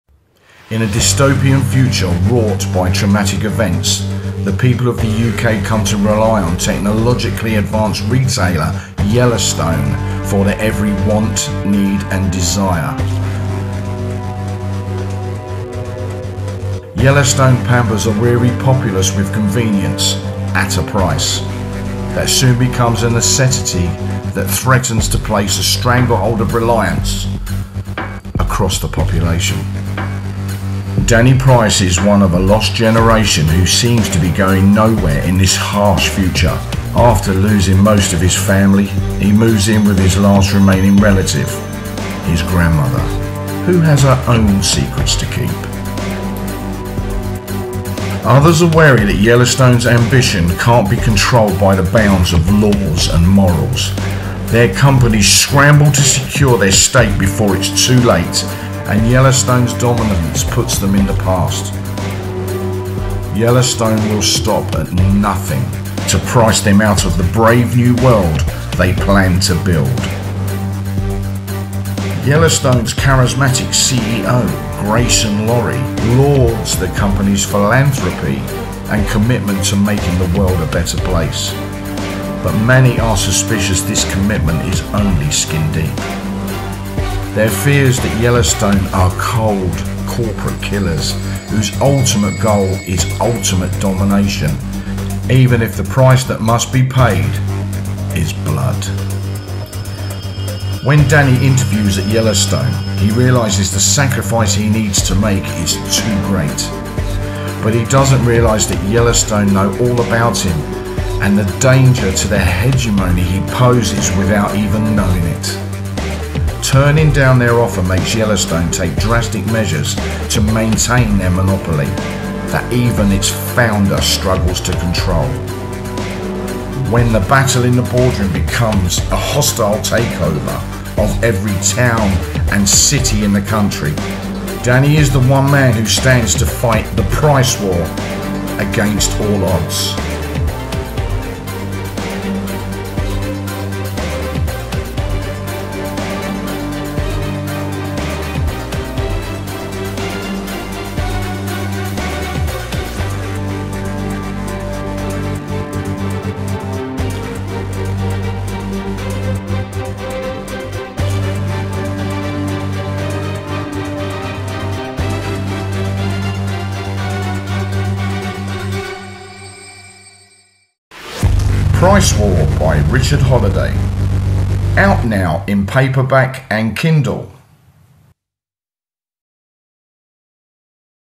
Price War | A cutting, action-packed technothriller | Book Trailer | Richard Holliday
When the cost of doing business is blood, one man must save what's left of his family from paying the price...

PRICE WAR is the new cutting, action-packed technothriller by Richard Holliday - out now in paperback, Kindle and Kindle Unlimited!

Voiceover by John Bagenal
Voiceover directed by Gary Thomas